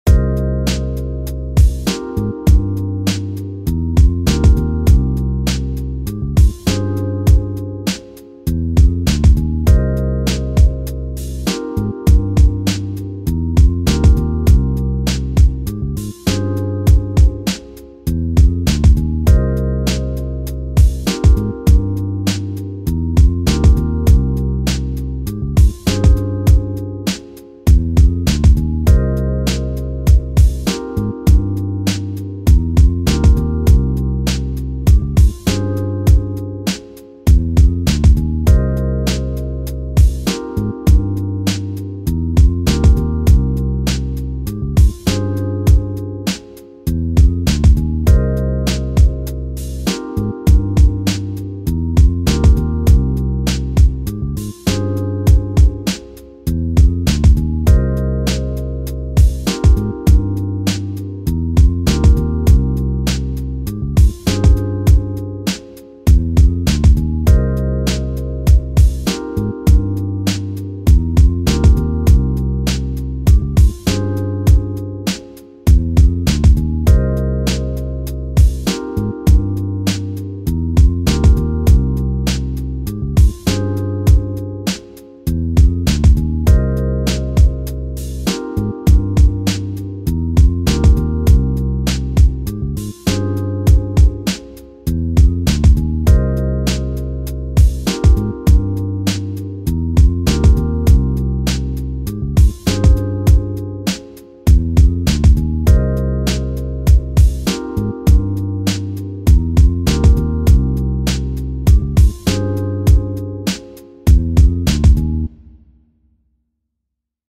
Dope Hip hop beat - blues jazz boom bap style
I have left the hook out so no one can steal my song. PM me for full version.